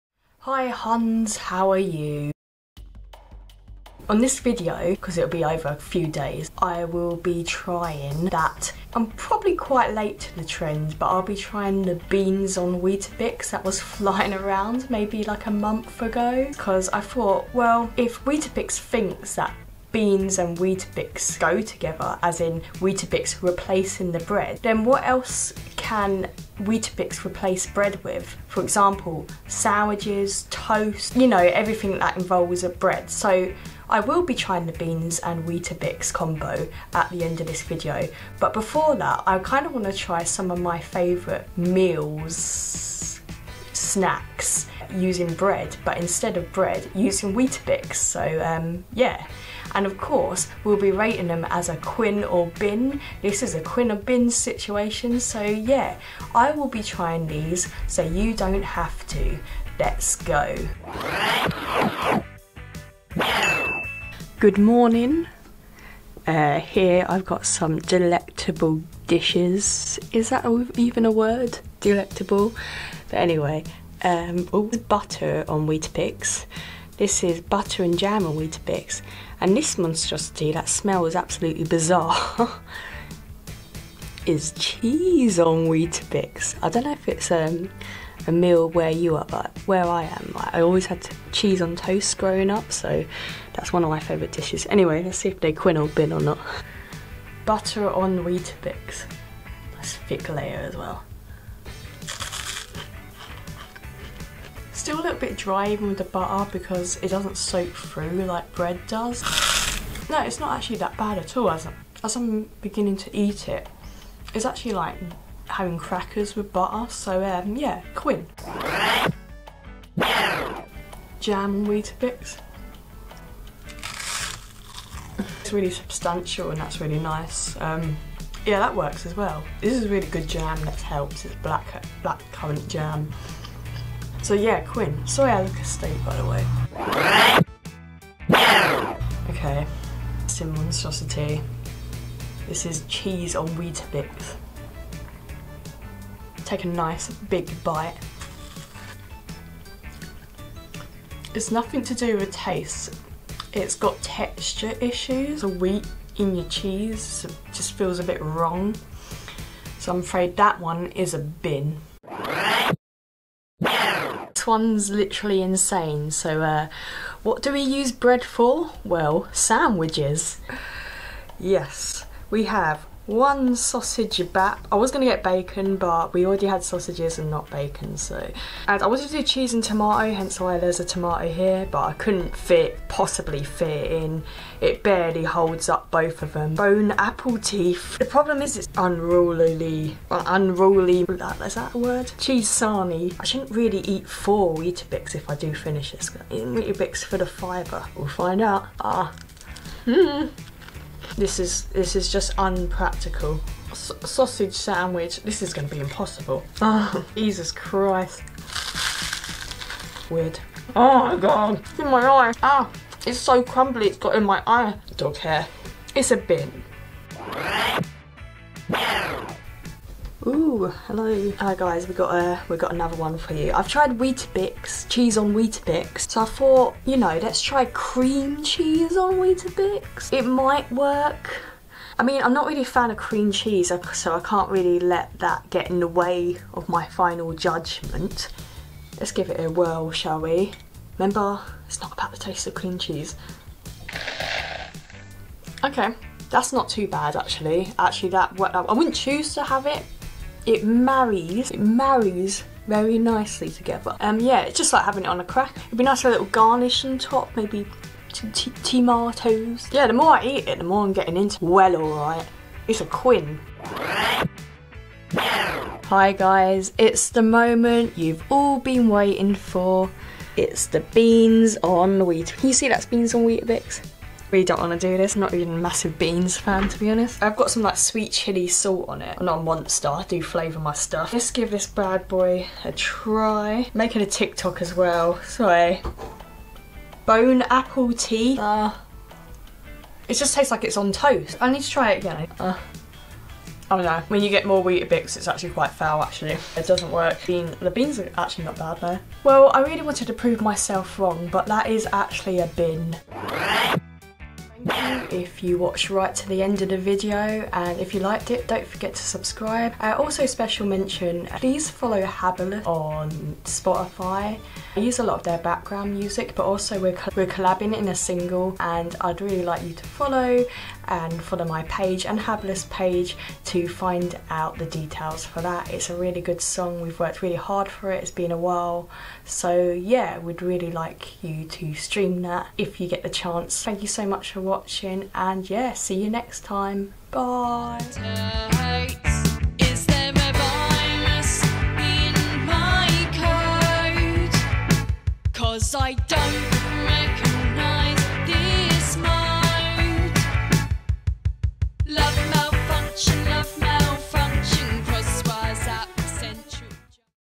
TRYING TRENDING BEANS ON WEETABIX (and other recipes with weetabix)
Mel Quinn contacts:
Music youtube channel: Mel Quinn music
x